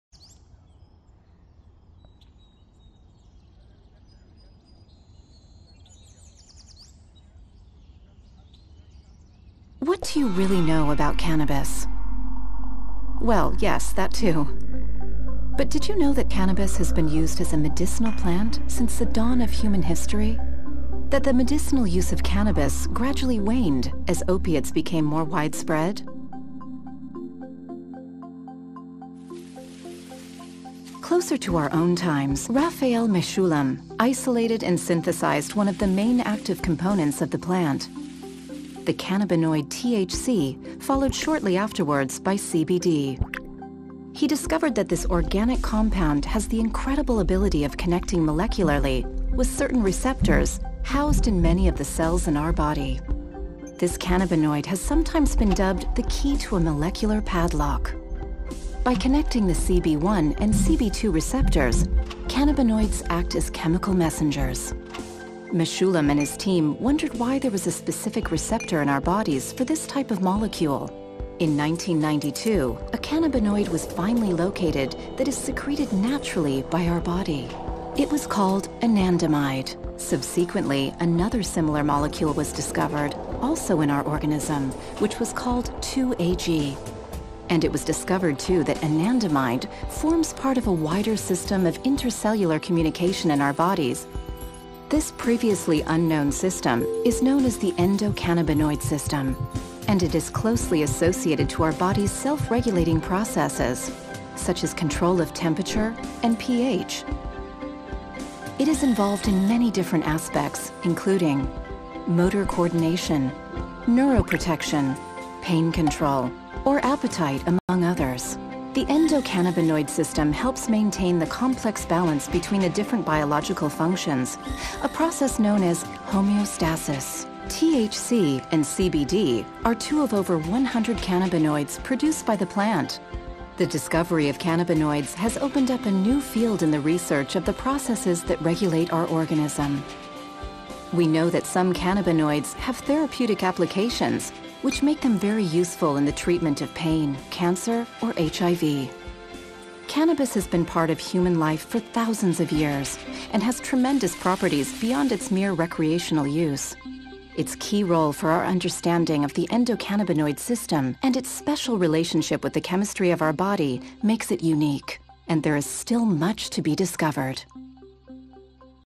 Endocannabinoid system - German subtitles
The endocannabinoid system is one of the great unknowns of the biology of our organism. It basically is a neurotransmission system, although it is much more than that, as it can also be found in other organs and body tissues besides the brain.

At Fundación CANNA we have developed a video to help you understand, in a simple way, what the ECS is and what its effects are at our internal functions. We also show the clear relation between the plants produced cannabinoids and how our body responds to that.